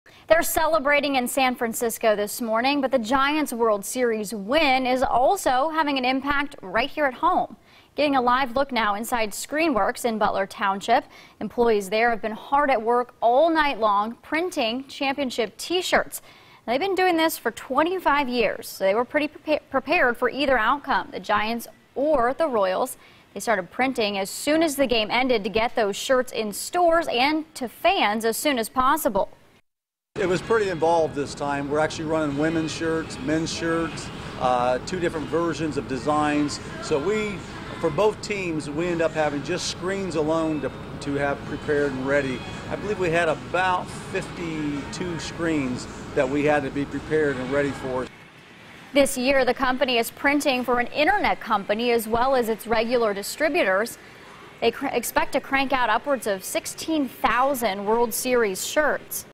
Local printing company pulls all-nighter
Screen Works is printing World Series championship t-shirts.